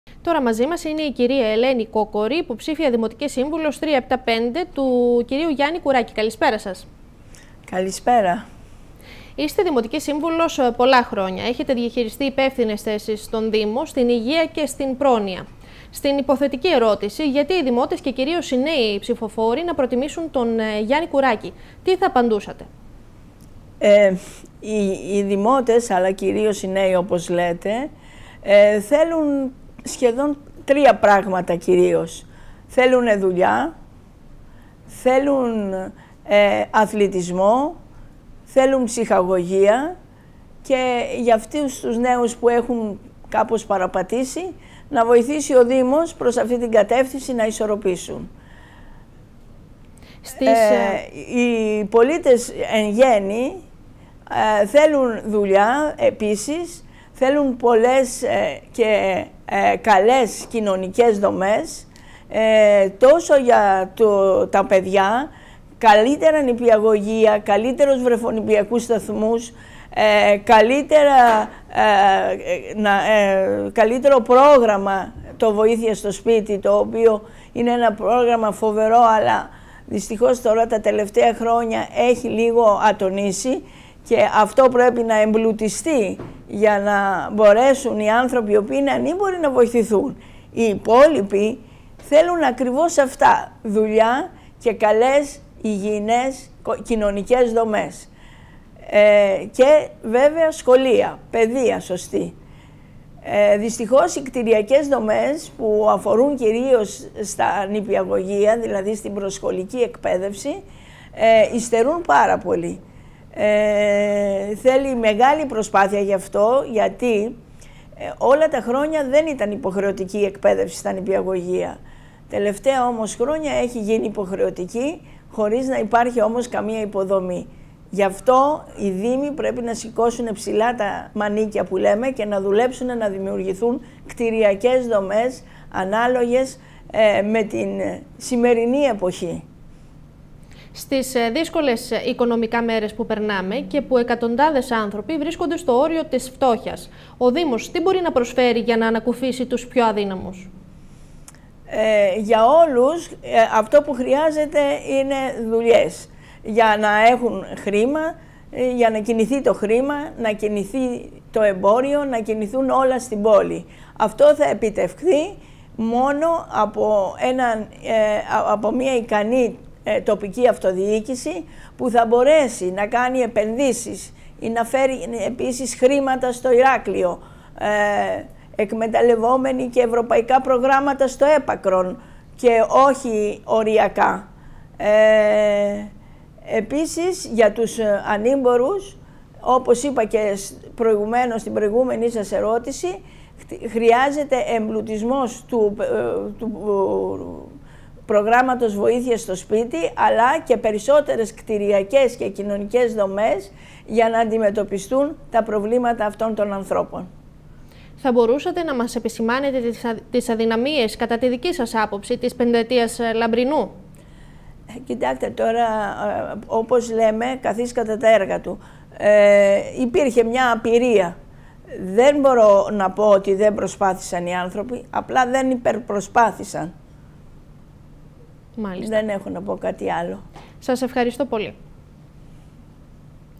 NotosNews - Ελένη Κοκκόρη
Καλεσμένη στο δελτίο ειδήσεων του notosTv η υποψήφια δημοτικός σύμβουλος ηρακλείου με το ''3.7.5.'', Ελένη Κοκκόρη